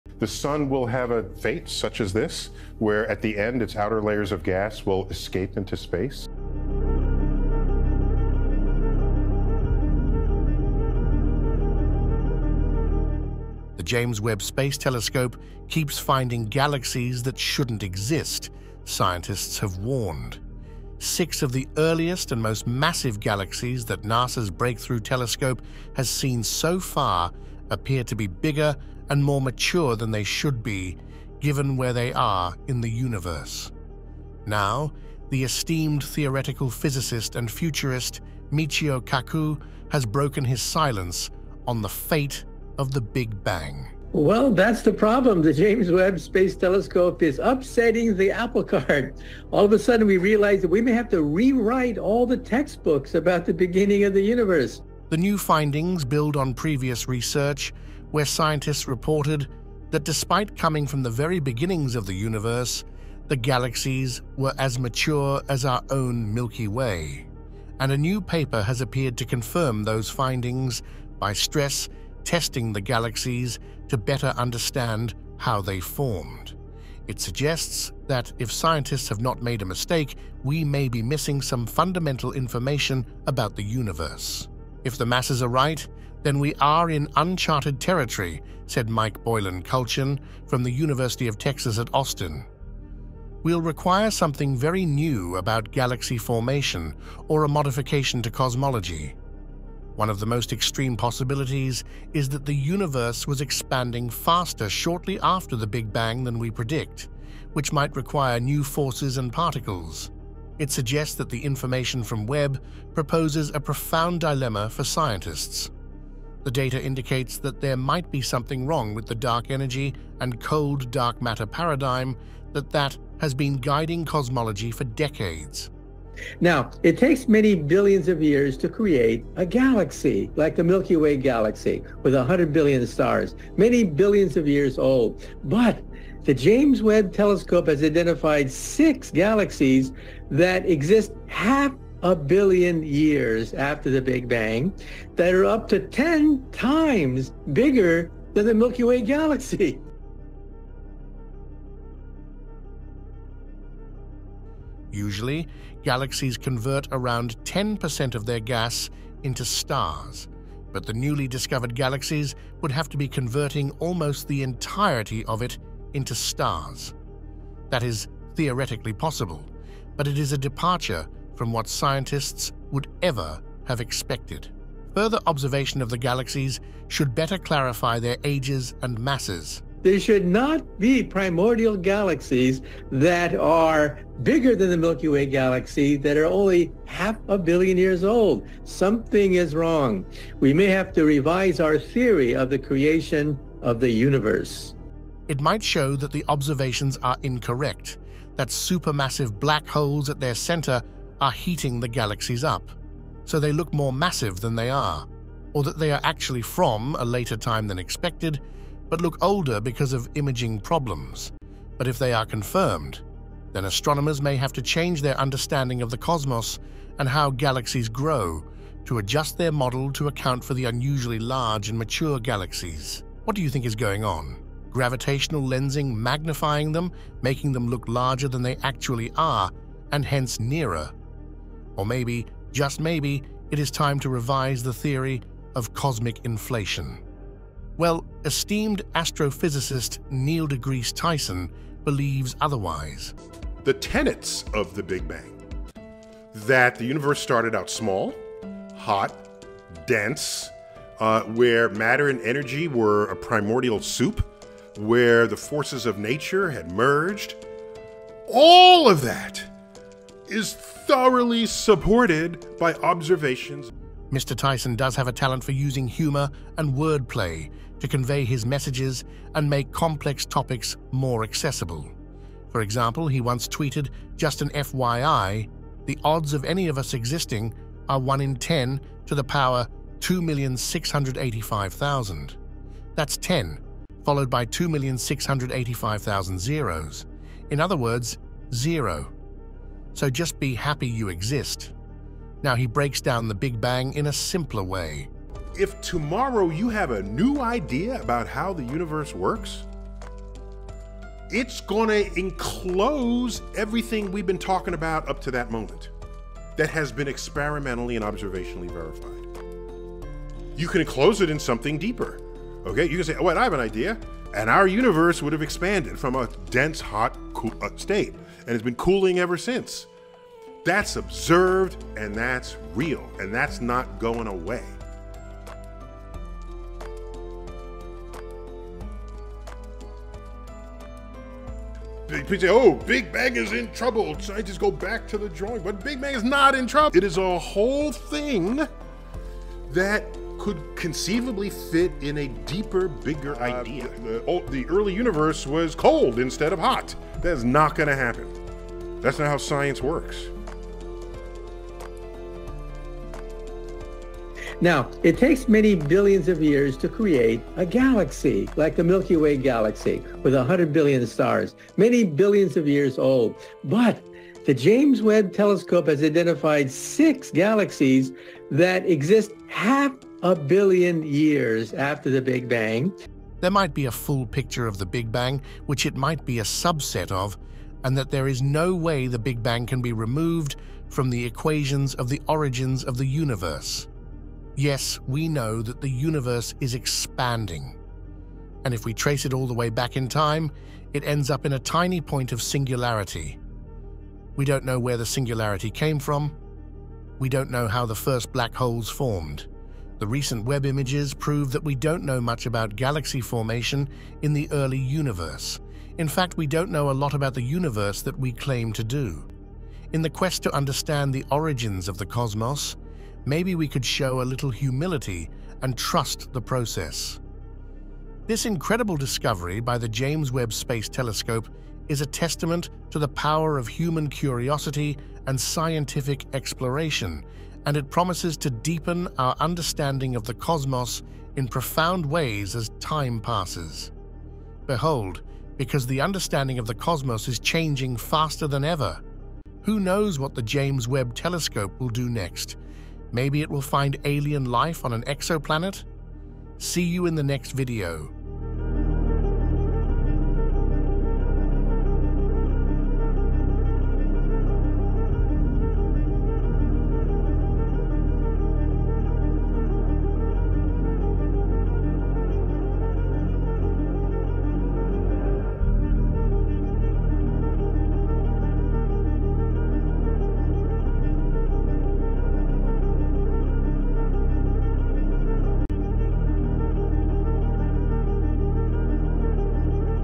"This is Impossible!" Neil deGrasse Tyson Breaks Silence On James Webb Telescopes Shocking New Image
Michio Kaku and Neil deGrasse Tyson weigh in on the latest image captured by the James Webb Space Telescope. A shocking discovery of ancient, massive galaxies, that forces the science community to rethink everything we know about the cosmos and our understanding of it. Yes, even the big bang theory.